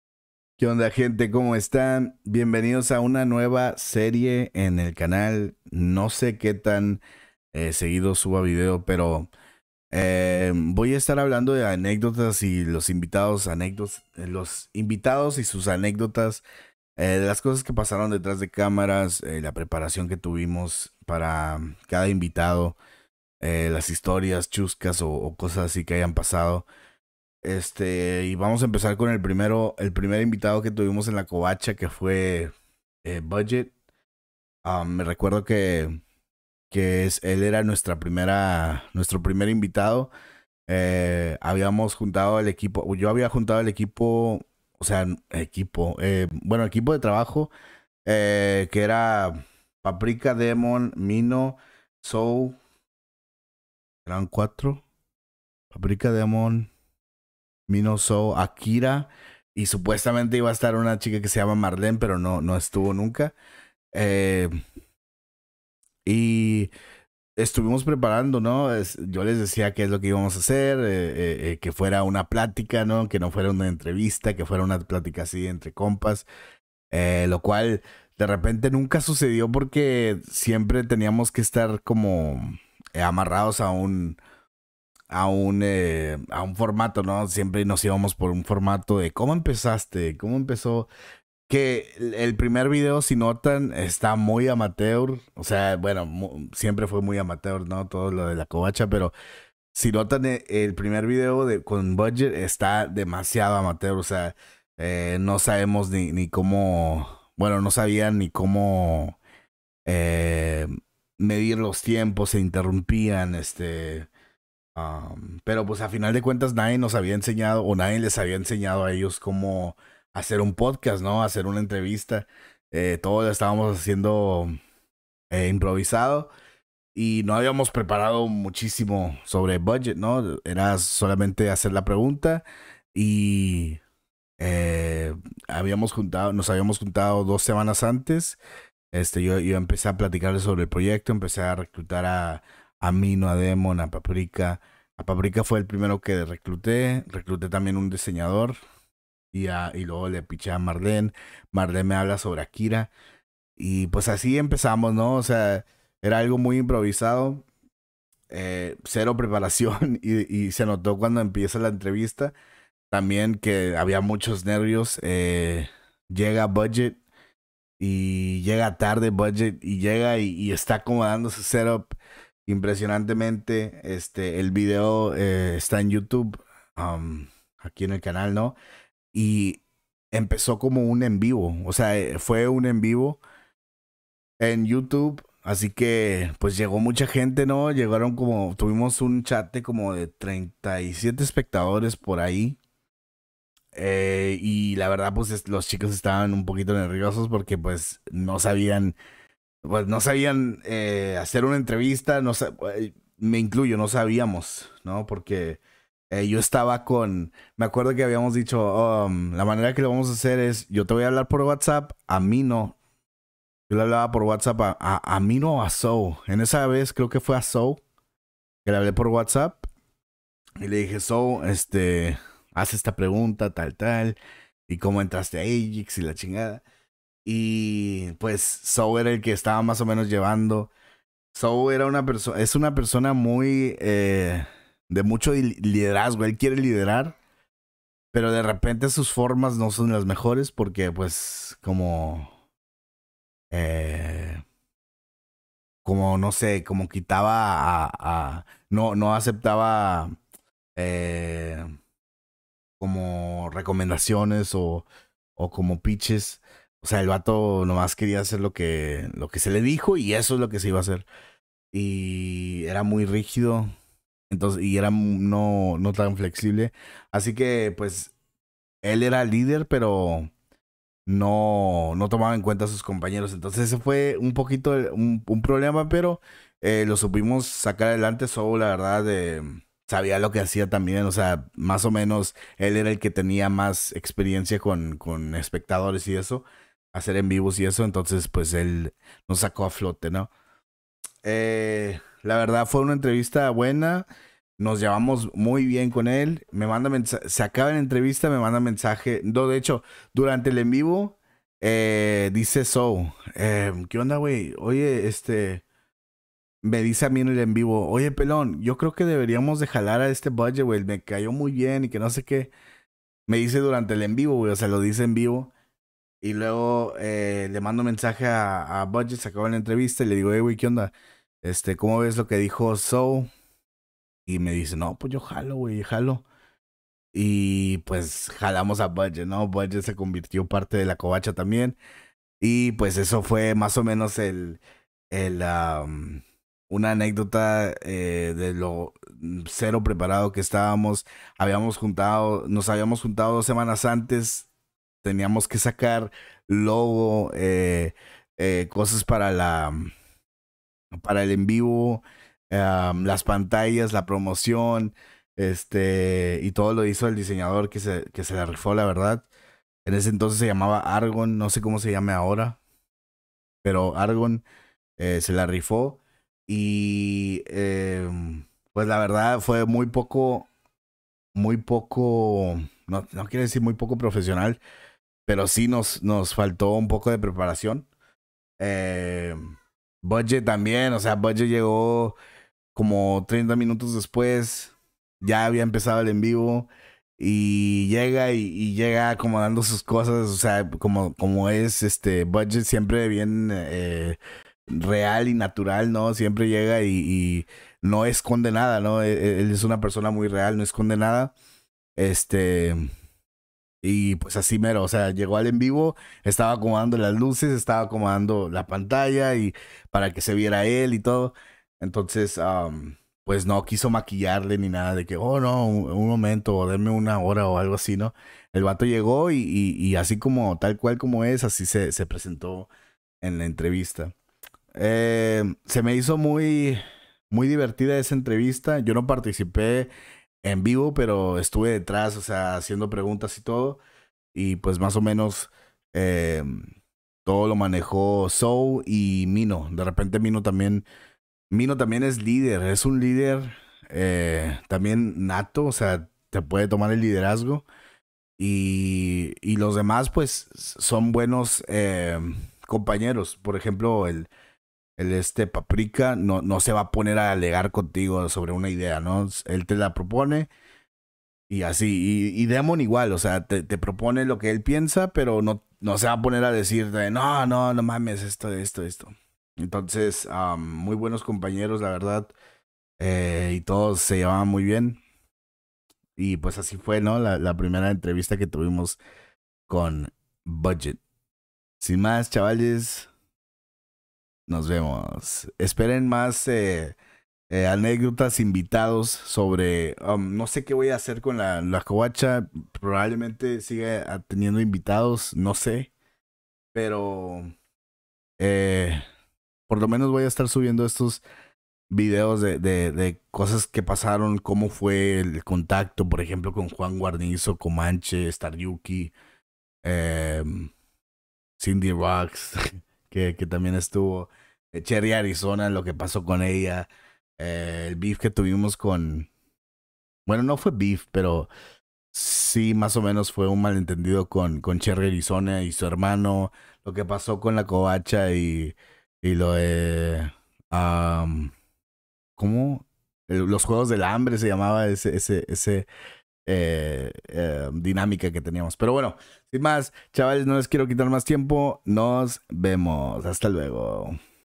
LA VERDAD DETRAS - DE LA ENTREVISTA A BUDGET - NO ESPERABAMOS ESTO DE EL
NUEVA SERIE PLATICANDO SOBRE LAS ANECDOTAS QUE MARCARON LAS ENTREVISTAS A NUESTROS INVITADOS HERMOSOS